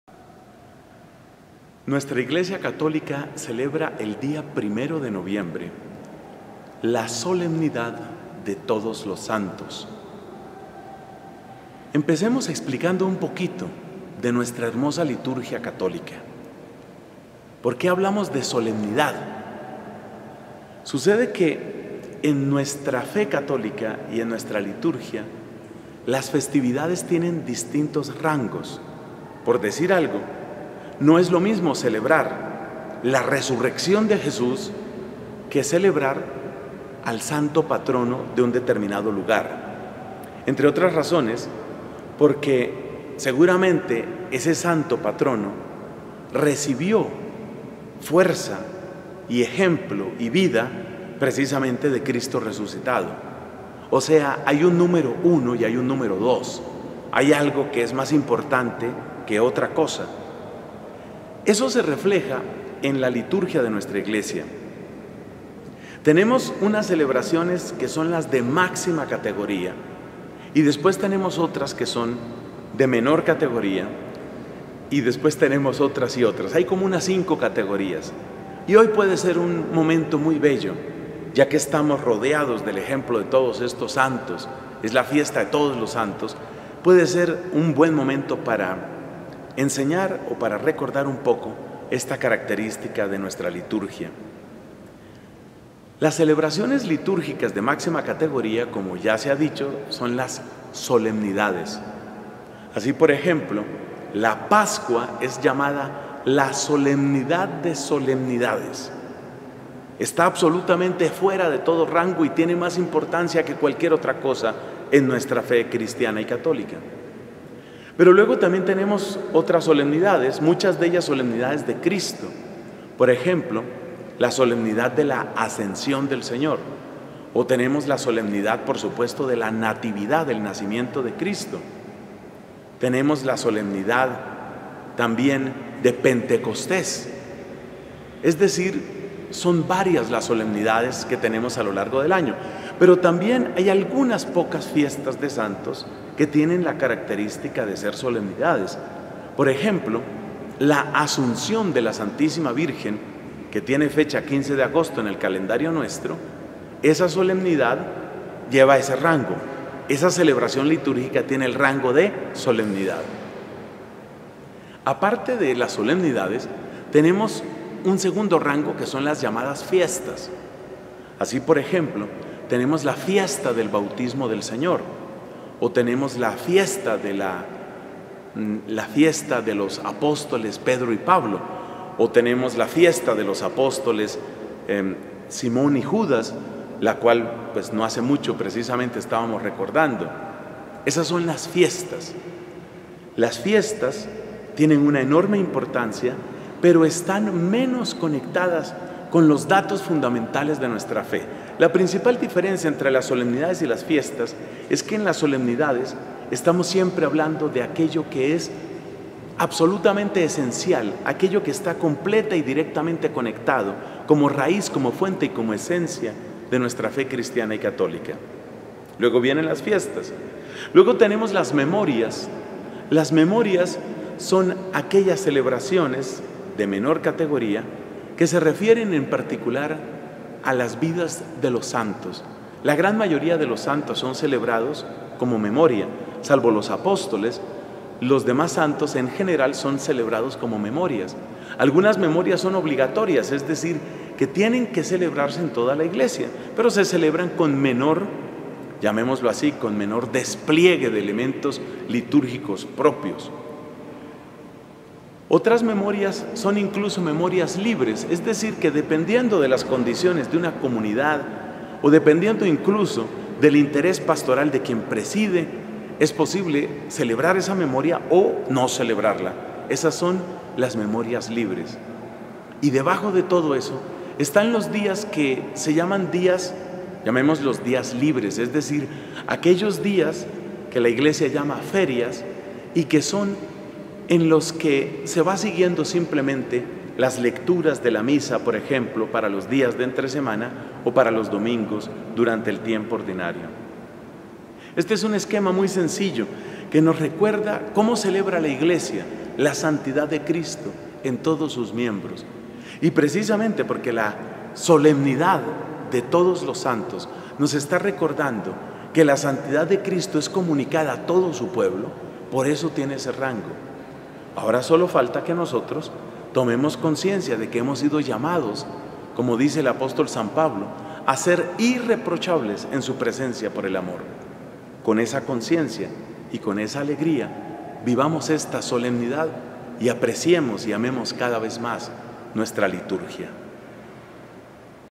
LA GRACIA del Sábado 1 de Noviembre de 2014
SOLEMNIDAD DE TODOS LOS SANTOS

Catequesis sobre los grados de celebración litúrgica en la Iglesia Católica.